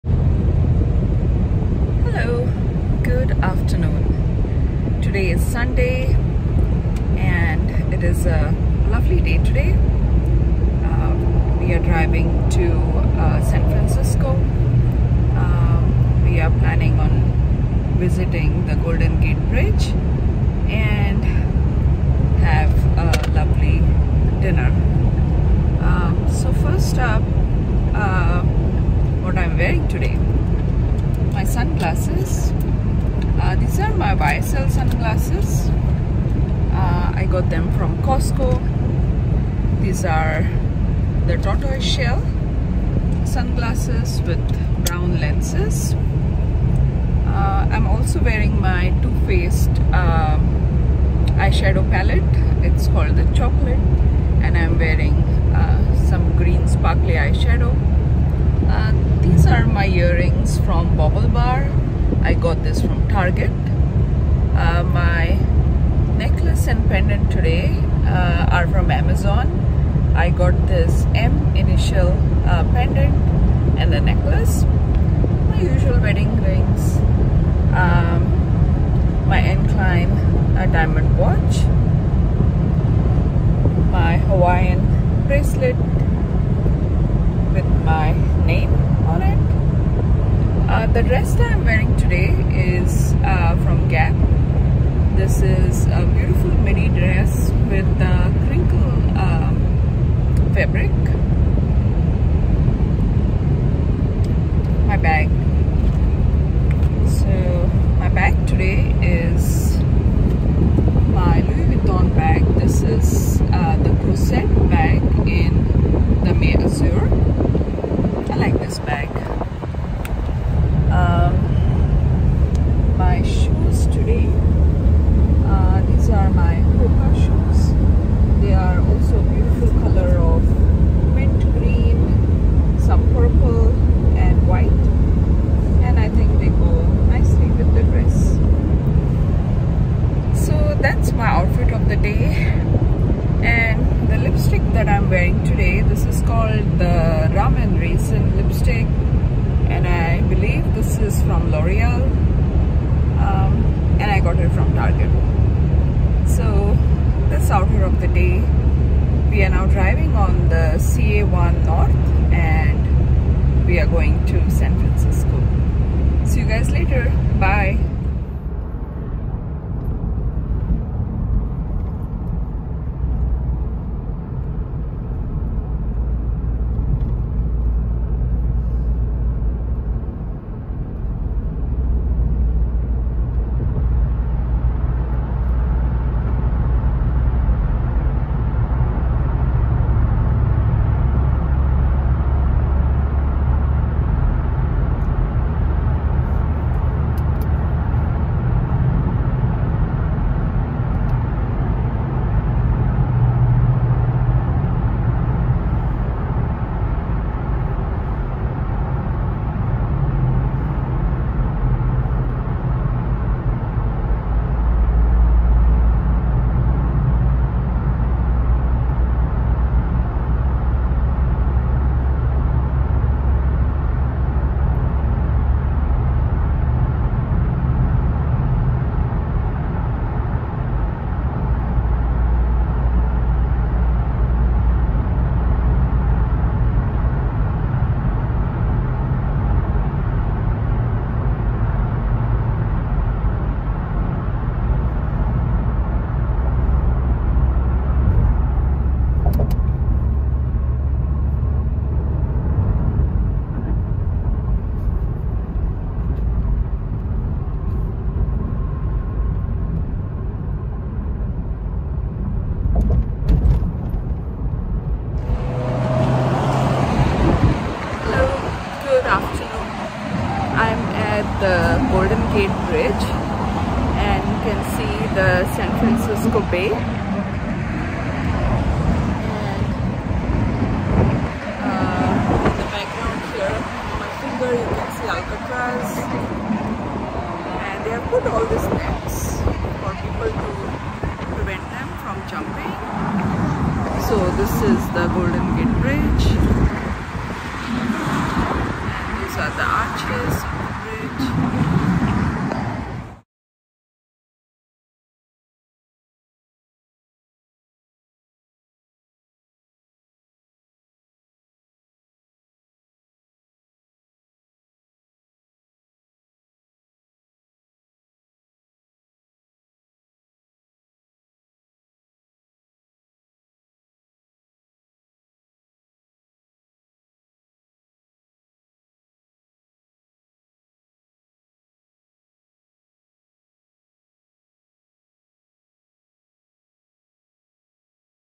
Driving to the Golden Gate Bridge, San Francisco, California
Hello! Join me on a drive to the Golden Gate Bridge in San Francisco. Later we had dinner followed by ice cream sundae at Ghirardelli.